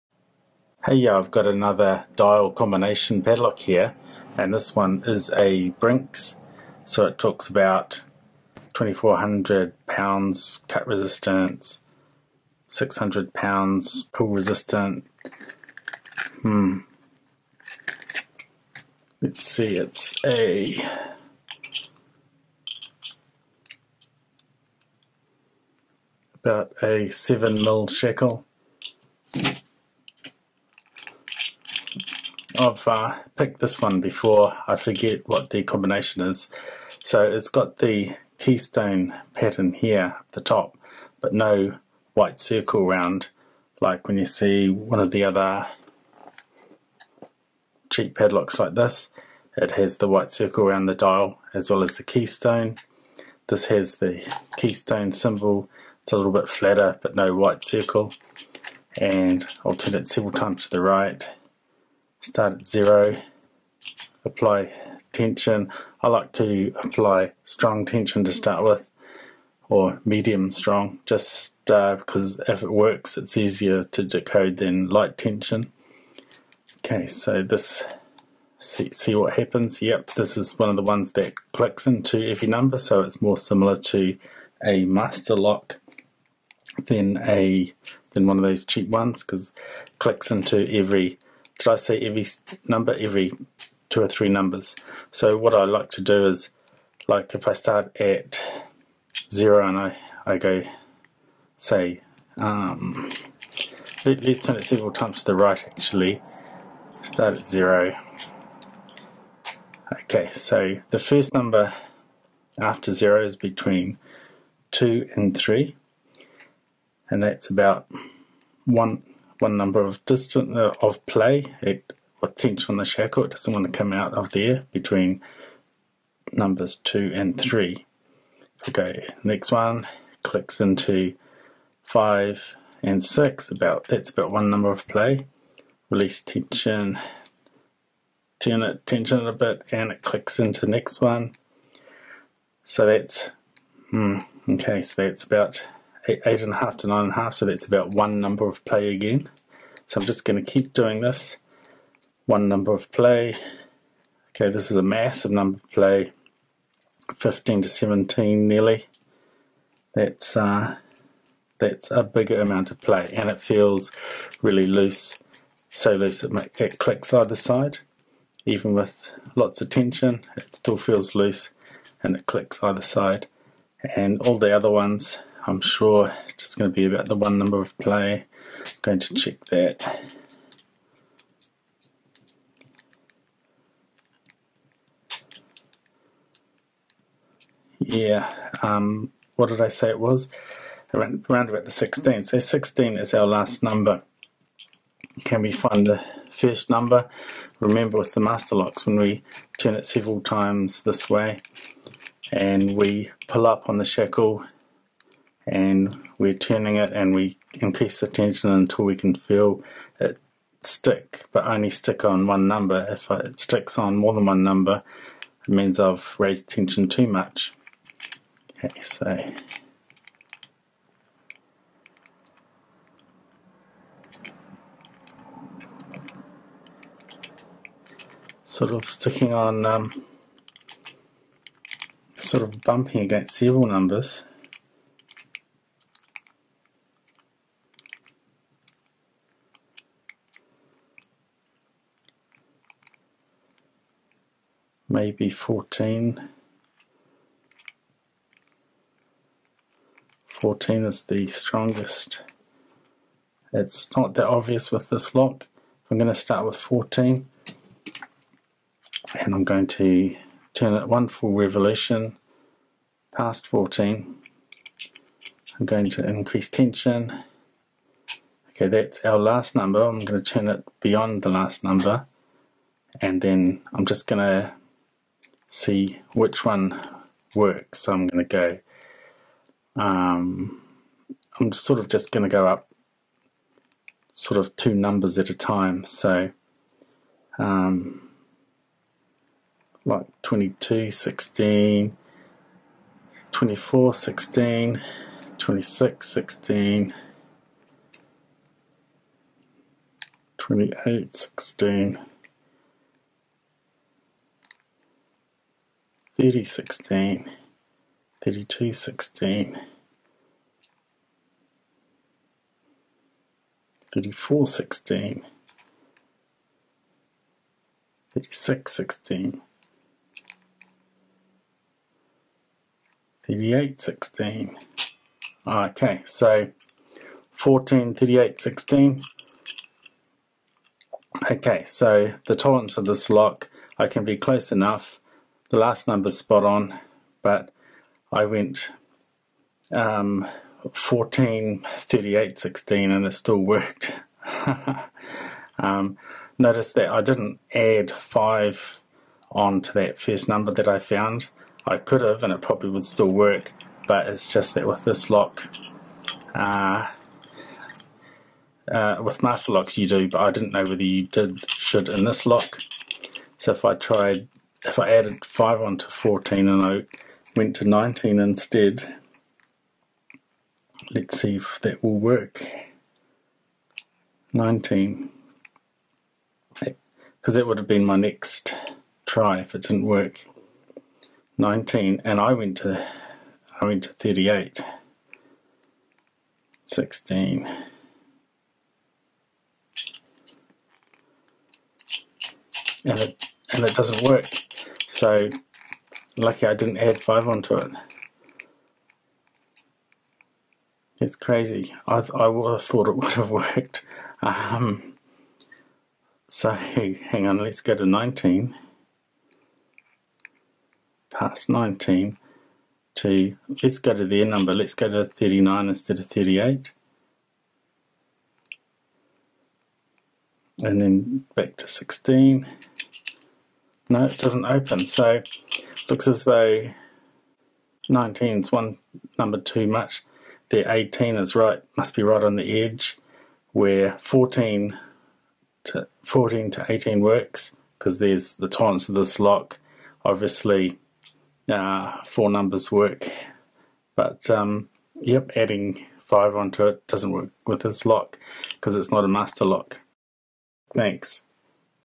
(Picking 104) How to crack a Brinks dial combination padlock
Here I decode a Brinks (model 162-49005) dial combination padlock.
You can try this method to unlock your padlock if you've lost your combination.
Note that this lock can't be shimmed open - I've tried!
I've done quite a few dial combo locks so for this one I skimmed over some of the details.